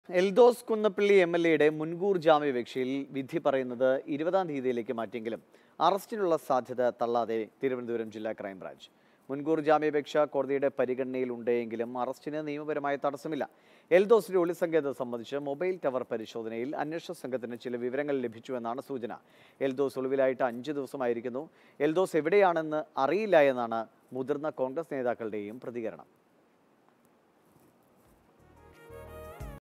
എൽദോസ് കുന്നപ്പിള്ളിയുടെ അറസ്റ്റിനുള്ള സാധ്യത തള്ളാതെ പോലീസ് | Mathrubhumi News
എൽദോസ് കുന്നപ്പിള്ളിൽ എംഎൽഎയുടെ മുൻകൂർ ജാമ്യാപേക്ഷയിൽ വിധി പറയുന്നത് 20 ലേക്ക് മാറ്റിയെങ്കിലും അറസ്റ്റിനുള്ള സാധ്യത തള്ളാതെ തിരുവനന്തപുരം ജില്ല ക്രൈം ബ്രാഞ്ച്.

Happy viewing!